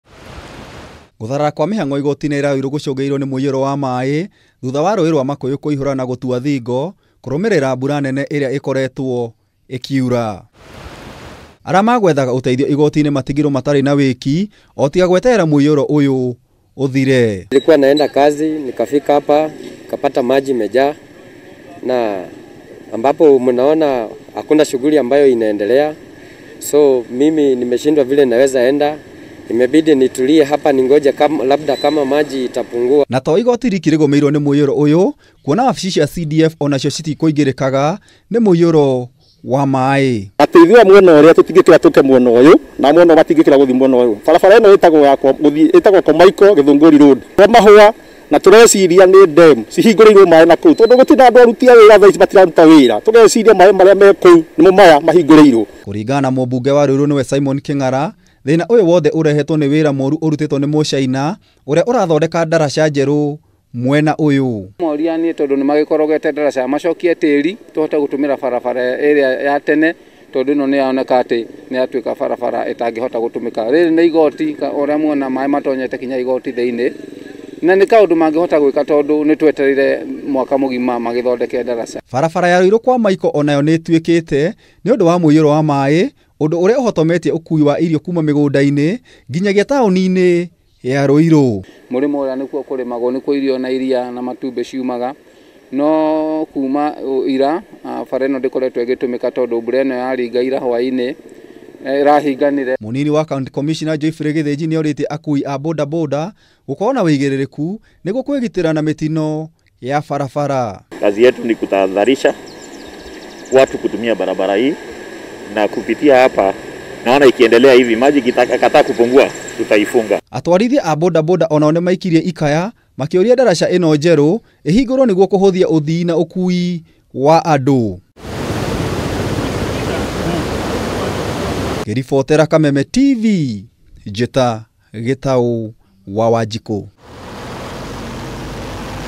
Mitangiko ya muiyuro Ruiru
Mirutire ya wira igotini ria Ruiru nii ithararirio ni muiyoro wa maii thutha wa mbura nene guthandana…
Mbura ino nii tigire barabara imwe iria cingiraga taonini ya Ruiru ithokangite ni muiyoro uyu kwogwo guthararia uthii…